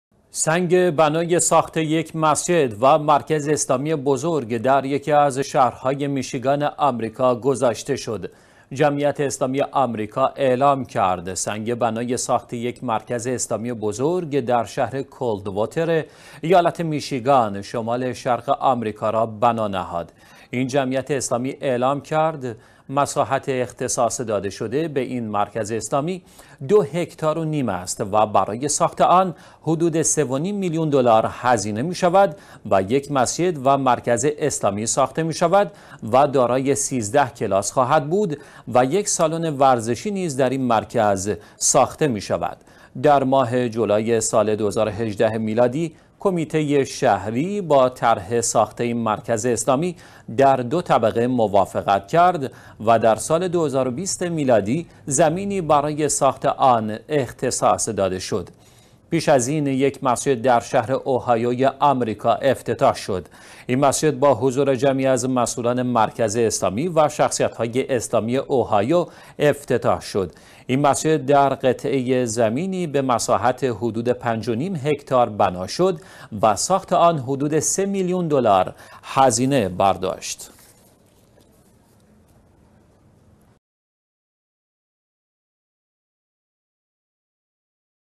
سنگ بنای ساخت یک مسجد و مرکز اسلامی در «میشیگان» گذاشته شد
سنگ بنای ساخت یک مسجد و مرکز اسلامی بزرگ در یکی از شهرهای میشیگان آمریکا گذاشته شد.
جمعیت اسلامی آمریکا اعلام کرد: سنگ بنای ساخت یک مرکز اسلامی بزرگ در شهر «کولدووتر» ایالت میشیگان شمال شرق آمریکا را بنا نهاد.
این جمعیت اسلامی اعلام کرد: مساحت اختصاص داده شده به این مرکزاسلامی دو هکتار و نیم است و برای ساخت آن حدود سه و نیم میلیون دلار هزینه می شود و یک مسجد و مرکز اسلامی ساخته می شود و دارای 13 کلاس خواهد بود و یک سالن ورزشی نیز در این مرکز ساخته می شود.
در ماه جولای سال 2018 میلادی کمیته شهری با طرح ساخت این مرکز اسلامی در دو طبقه موافقت کرد و در سال 2020 میلادی زمینی برای ساخت آن اختصاص داده شد.
پیش از این، یک مسجد در شهر اوهایو آمریکا افتتاح شد. این مسجد با حضور جمعی از مسئولان مرکز اسلامی و شخصیت های اسلامی اوهایو افتتاح شد. این مسجد در قطعه زمینی به مساحت حدود پنج و نیم هکتار بنا شد و ساخت آن حدود سه میلیون دلار هزینه برداشت.